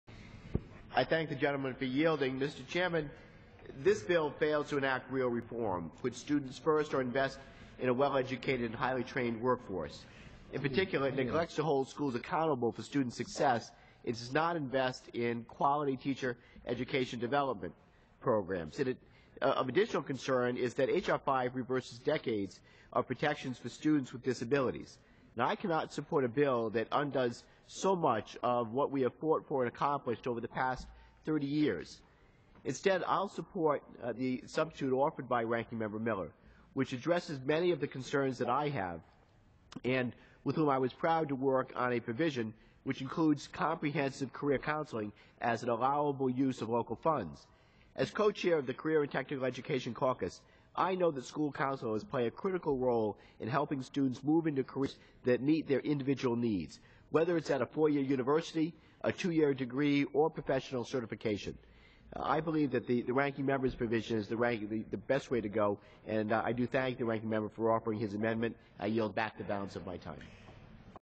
The Student Success Act 7-18-13
Rep. Langevin spoke on the House floor in opposition of the Student Success Act, stating that it does not enact real education reform.